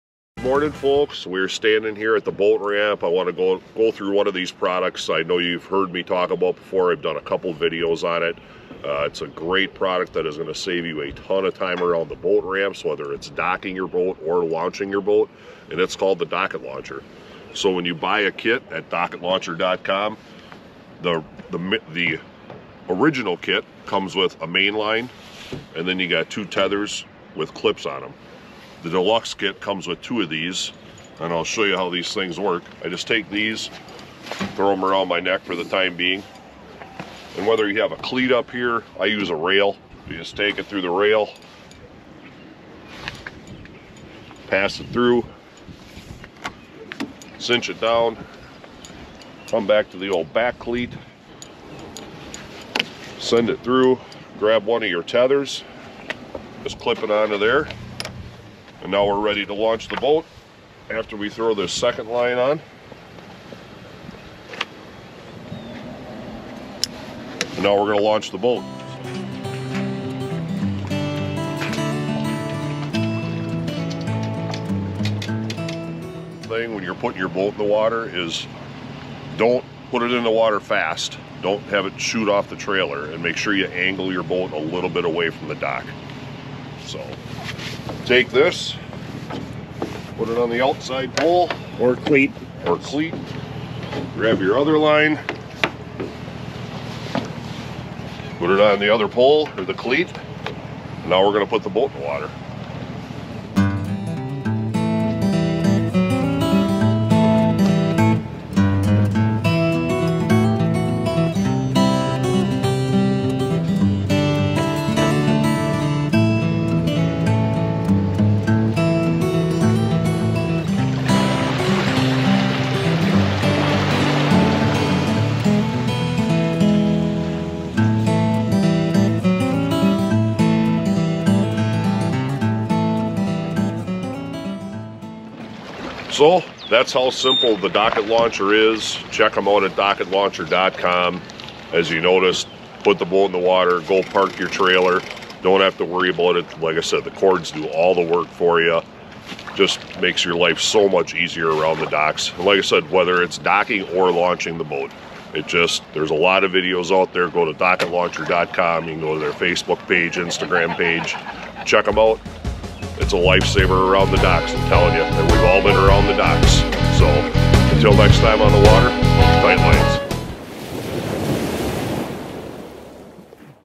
Launching by yourself with DOCK-IT LAUNCHER. See a demonstration with Burnsies' Guide Service.
Burnsies' Guide Service demonstrates a Dock-It launcher kit for solo-launching and docking. dock-itlauncher.com